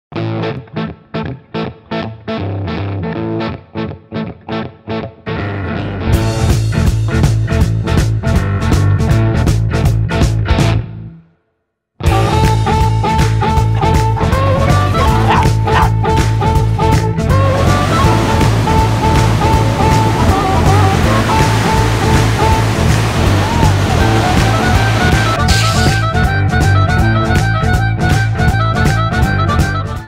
Surf Dogs Entry: 2013 Doritos Crash The Superbowl Contest RUNNER-UP
The surf dogs raise money to buy surf boards, but Tara "the mogul" realizes they've made enough money to buy a huge yacht.  As the boys hit the surf, Tara heads out on her new boat!